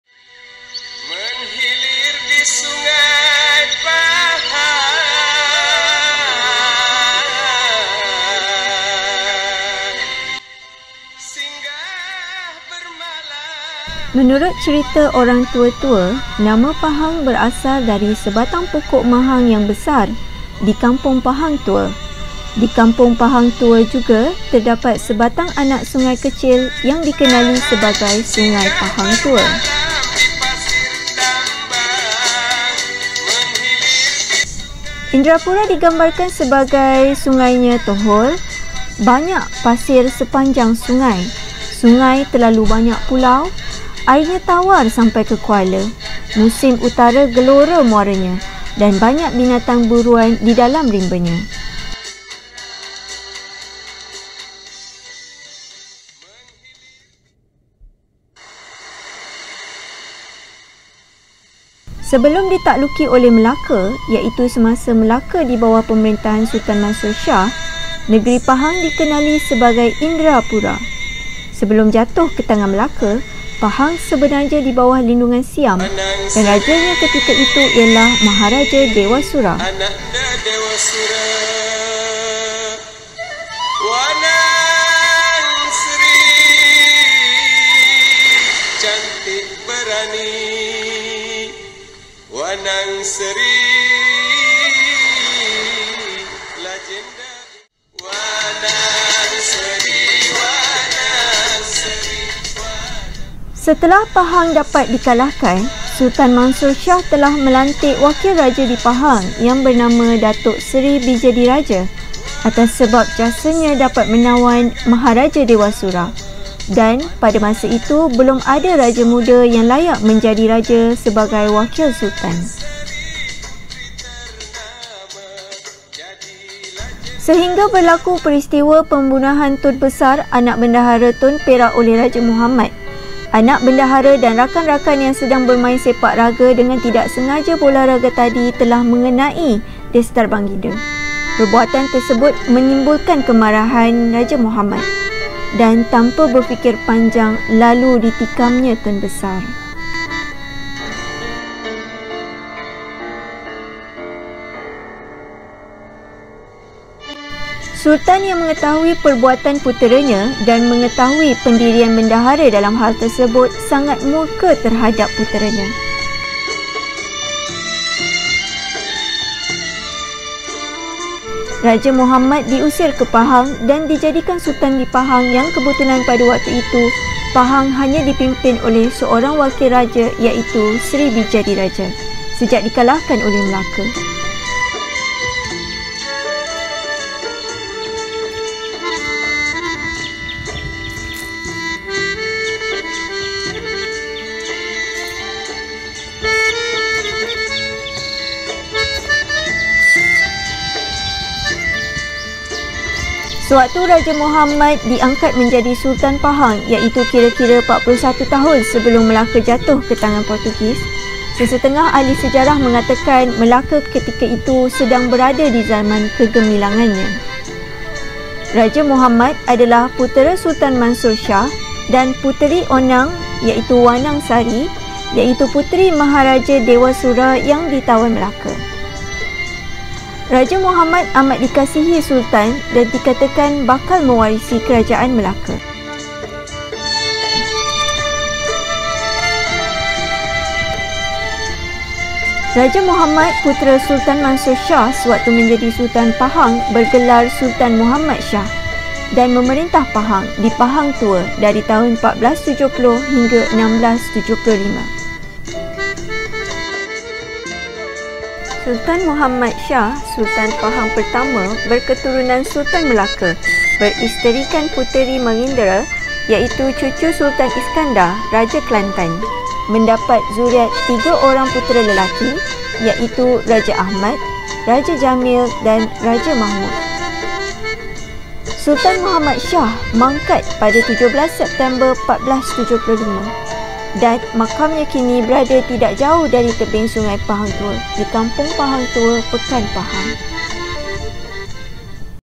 Sultan Muhammad Shah - Sultan Pahang Pertama Dari Keturunan Sultan Melaka (Sultan Mansur Shah)
Raja Muhammad putera Sultan Mansur Shah diangkat menjadi Sultan Pahang Pertama keturunan Sultan Melaka, setelah Pahang (Indera Pura) dialahkan Melaka.  Sebelum itu Pahang di bawah Siam dan wakil Siam di Pahang bergelar Raja Dewa Sura. dan Melaka di perintah oleh Sultan Mansur Shah. Peristiwa pembunuhan Tun Besar anak Bendahara Tun Perak oleh Raja Muhammad angkara bola raga. Punca pembunuhan terjadi bila Tun Besar bersama rakan-rakannya sedang bermain sepak raga dan tanpa disengajakan bola tersebut telah mengenai destar Raja Muhammad dan jatuh ke tanah. Raja Muhammad yang murka lalu menikam Tun Besar dan Tun Besar meninggal dunia ketika itu juga. Raja Muhammad telah di usir dari Melaka, oleh ayahandanya namun diangkat menjadi  Raja di Pahang bergelar Sultan Muhammad Shah, dan memerintah Pahang, di Pahang Tua pada tahun 1470 hingga 1475. Baginda mangkat di Pahang Tua dan makamnya sehingga kini berada tidak jauh dari Sungai Pahang Tua di Kampung Pahang Tua, Pekan, Pahang. Setelah baginda mangkat, adinda baginda iaitu yang juga beribukan Puteri Onang Sari diangkat menjadi Raja Pahang Ke-2 keturunan dari Raja Melaka.